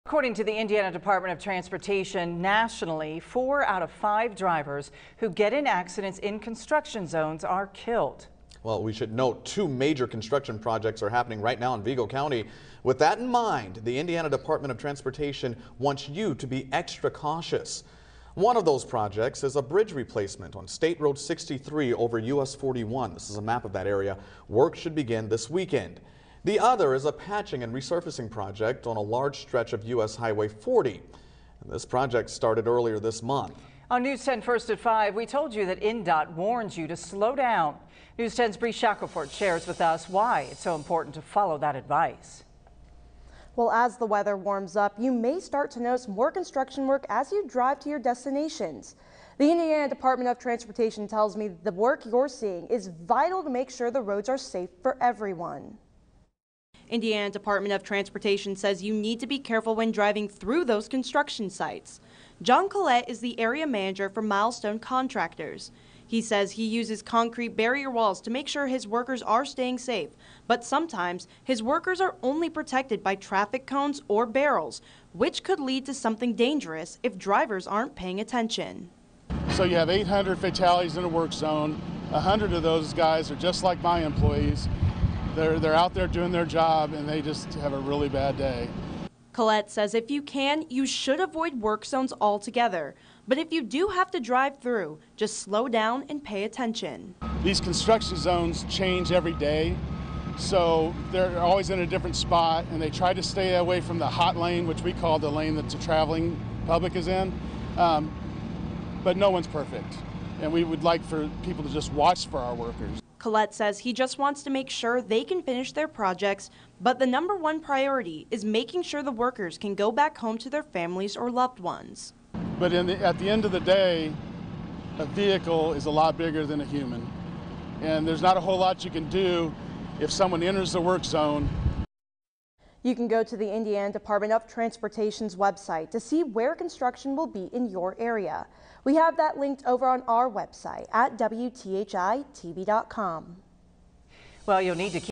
The Indiana Department of Transportation wants you to be careful when traveling in work zones
As the weather warms up more and more people are hitting the road. However, they're not the only ones out on the roads. It's road construction season and work zones are filled with crews tackling improvement projects.